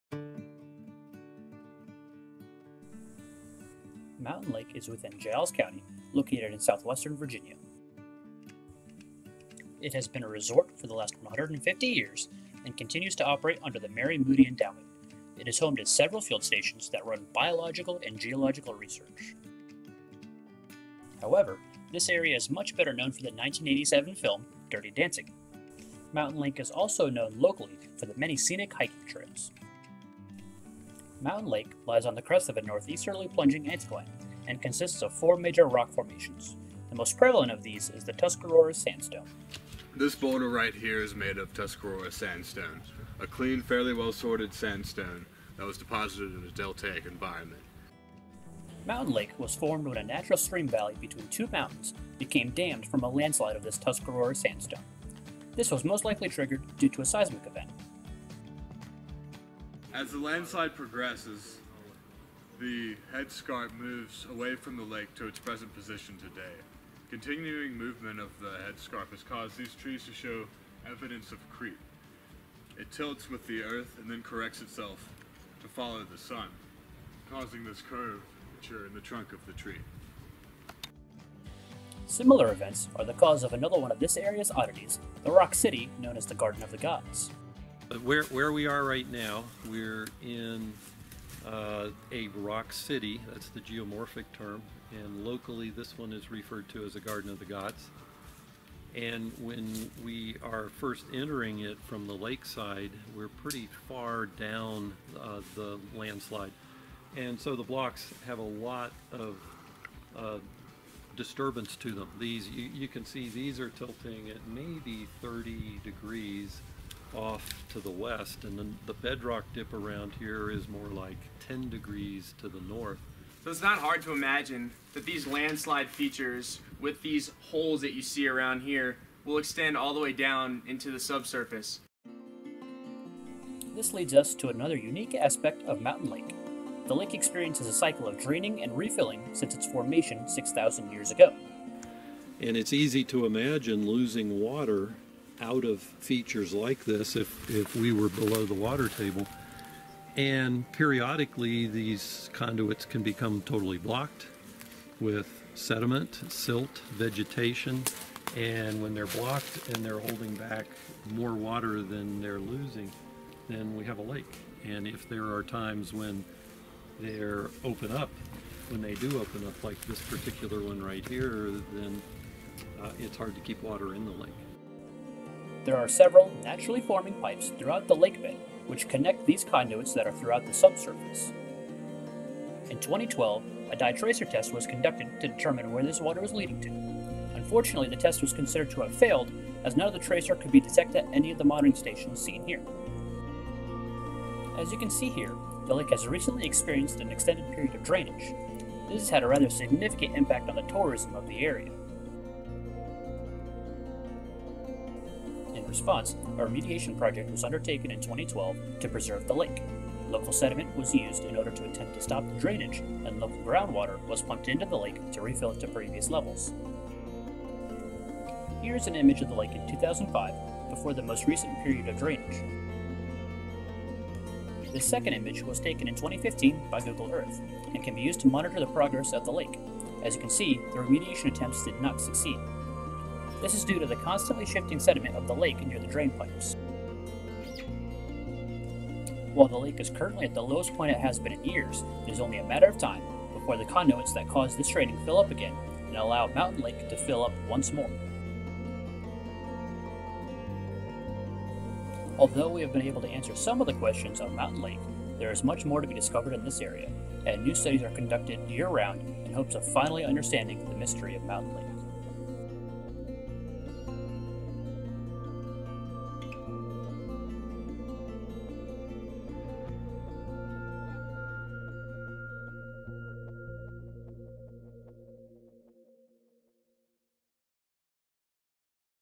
Geology of Mountain Lake
Virginia Tech Senior Seminar (Dr. Spotila, GEOS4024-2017) student-created informational video of the geology of Mountain Lake, Virginia.